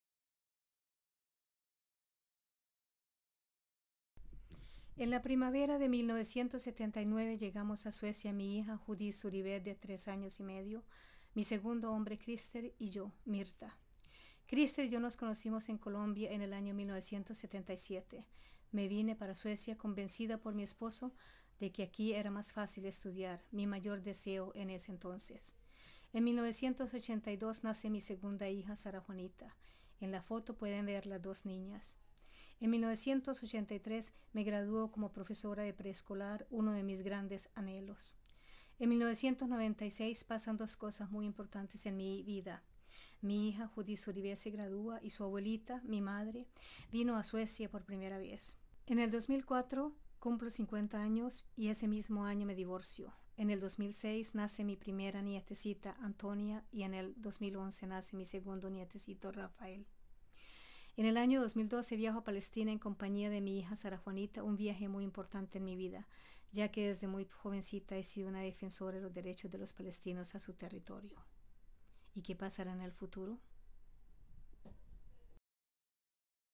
Så började mitt liv i Sverige
Denna digitala berättelse av Mirtha Murillo är gjord på en workshop i digitalt berättande på tema migration anordnad av Riksantikvarieämbetet med Kulturskolan Stockholm. Se vidare